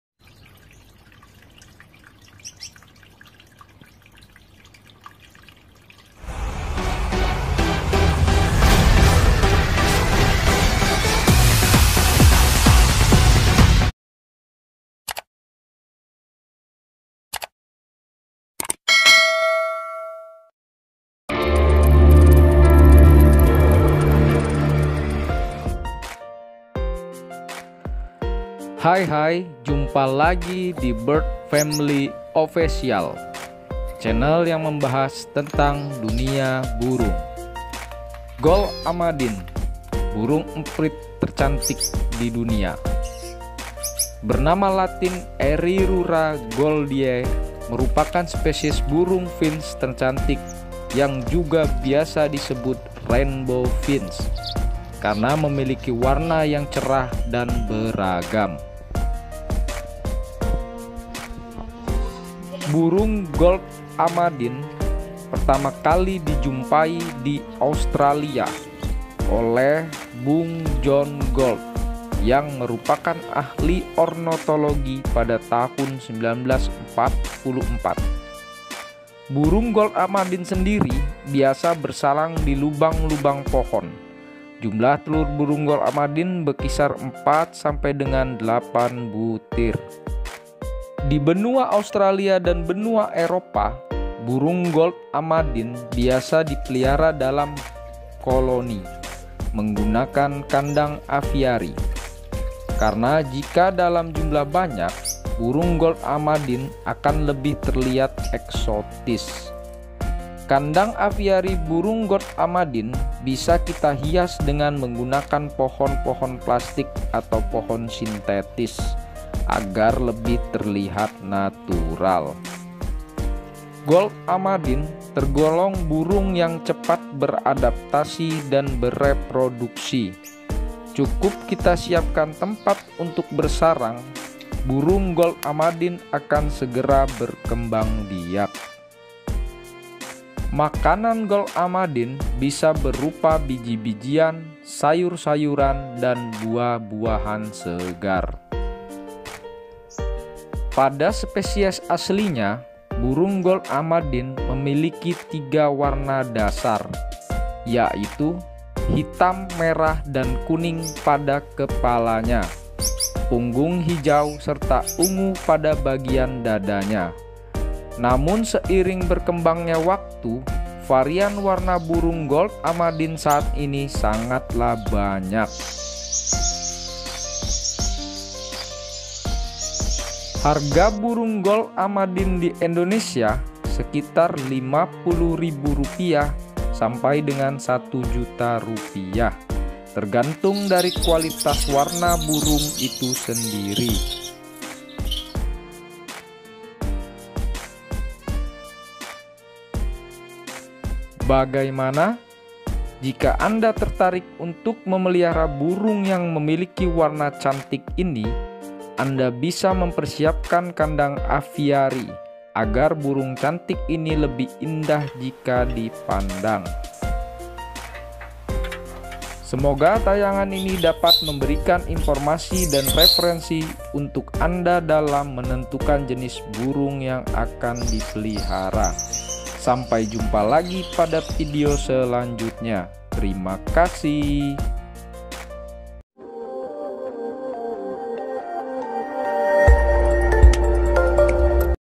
burung Finch paling yang lucu dan meriah ➡️ Gould amadin
Gould amadin adalah jenis burung emprit tercantik

memiliki warna cerah dan beragam burung gould amadin memiliki daya tarik tersendiri

burung emprit tercantik ini gampang beradaptasi dan berkembang biak

ayo simak video burung emprit tercantik sini semoga  menambah wawasan dan dapat menghibur

- (music/images/overlays/visum effects/etc.) courtesy of kinemaster corporation